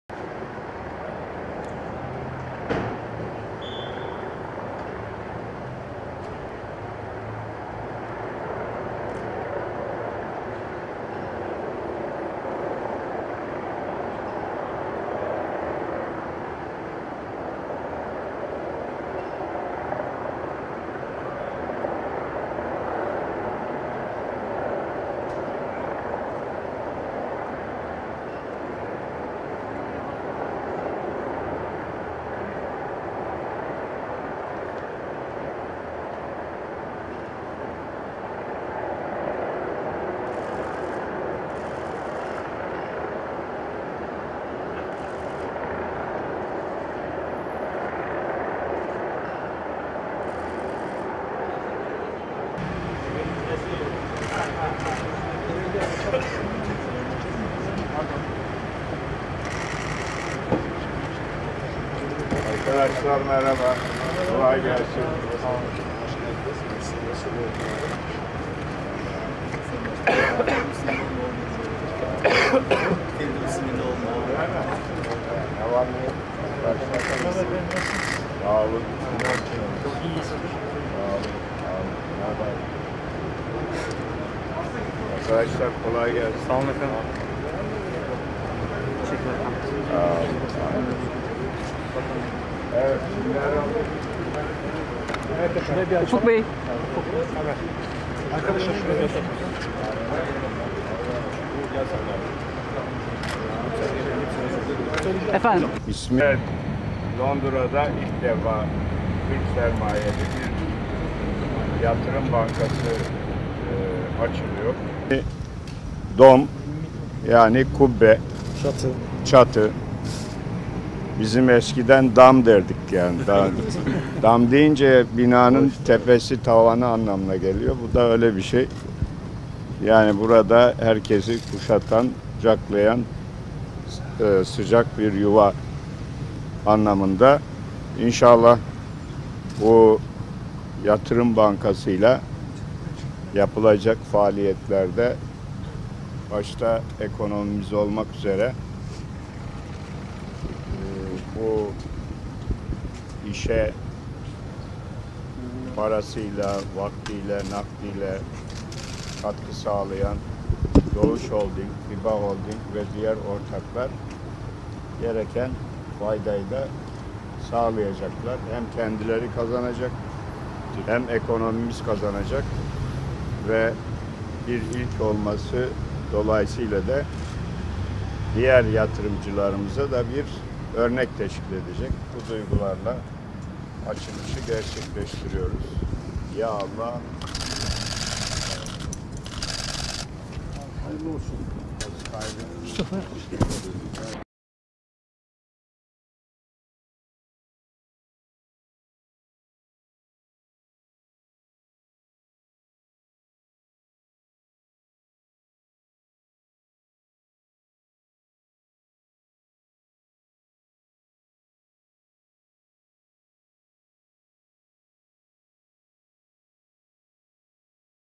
Başbakan Yıldırım İngiltere'de
Başbakan Binali Yıldırım, Londra'daki ilk Türk sermayeli "DOME Group" yatırım bankasının açılışını yaptı.

Yıldırım, yatırım bankasının açılışında yaptığı konuşmada, "dome" kelimesinin Türkçe karşılığının "kubbe", "çatı" ve "kuşatan" anlamına geldiğini söyledi.